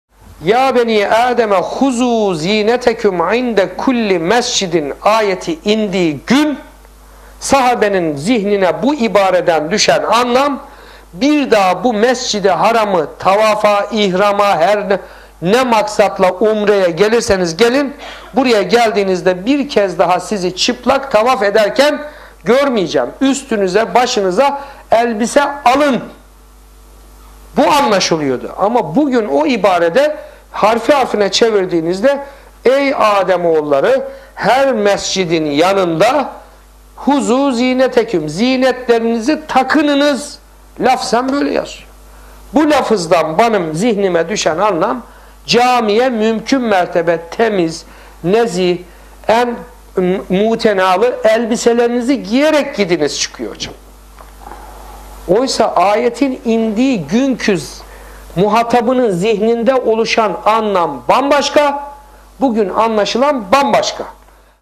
Araf 31 Ayetinin Doğru Çevirisi - Prof.Dr. Mustafa Öztürk
A'raf 31 (Mescide Her Gittiğinizde Ziynetlerinizi Takının) Ayetinin Doğru Çevirisi - Prof.Dr.Mustafa Ozturk